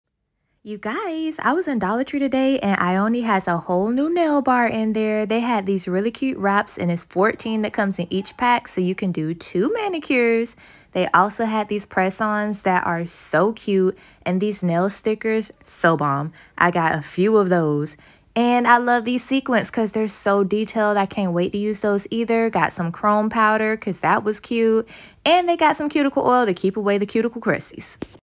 DOLLAR TREE’S NEW PRODUCT LINE! 🤯 5 must buy nail items you need cheaper than AMAZON
Hi WAM FAM! Today I’m showing you what Dollar Tree is rolling out for their new Ioni nail bar collection. They have nail wraps, press on’s, nail stickers, chrome powder and more! To top it off they are way CHEAPER THAN AMAZON! Run don’t walk to DOLLAR TREE!